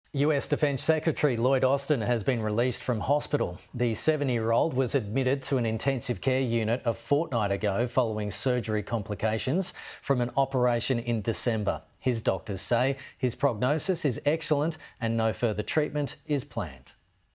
US Defence Secretary released from hospital
US Defence Secretary Lloyd Austin has been released from hospital.

The 70-year-old was admitted to an intensive care unit a fortnight ago.

His admission followed surgery complications from an operation in December.

His doctors claim his prognosis is excellent.

No further treatment is planned for Mr Austin, according to his doctors.